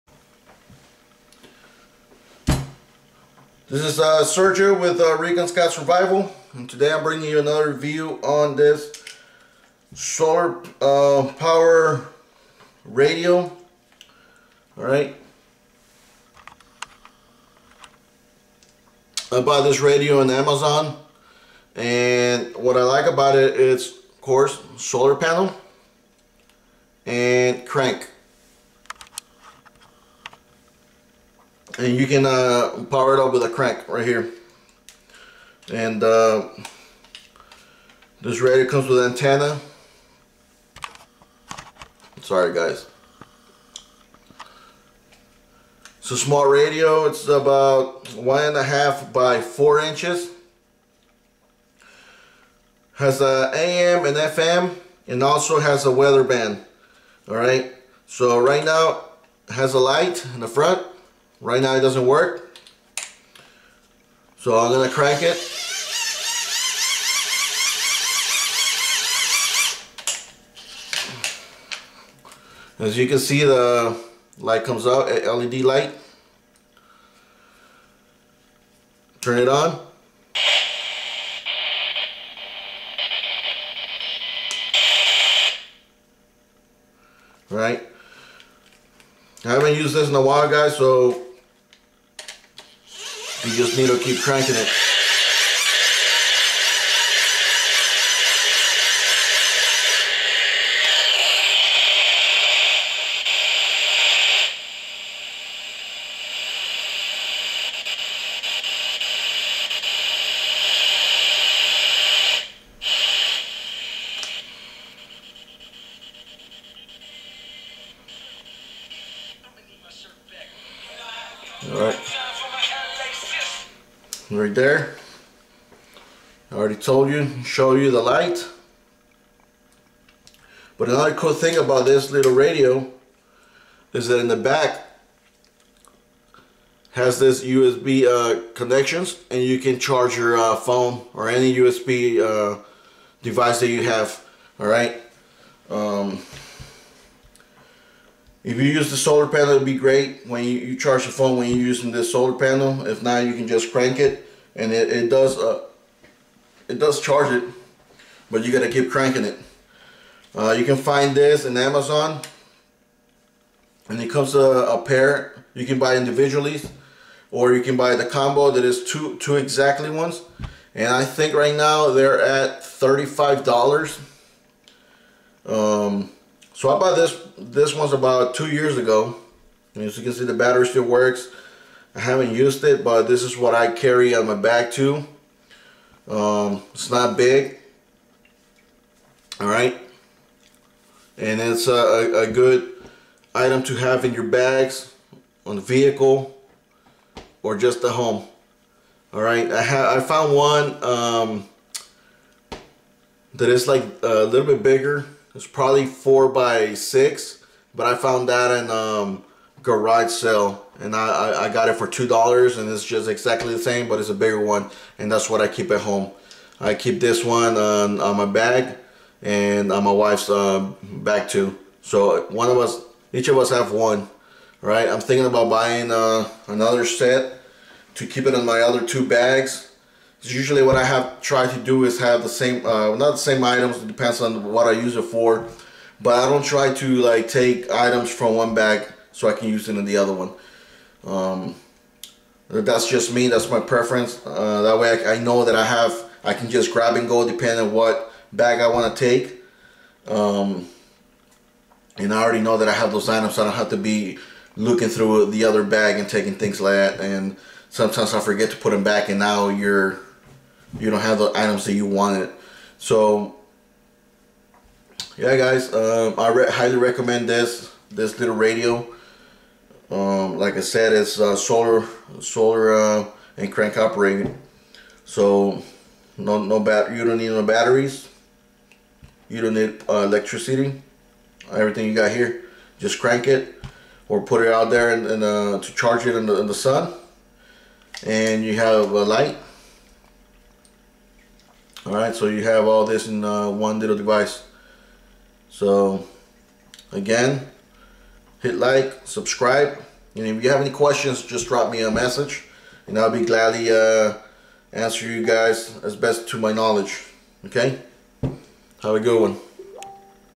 Solar and crank radio
Solar and crank radio. Has AM/FM/WB, LED light and you can charge your usb devices.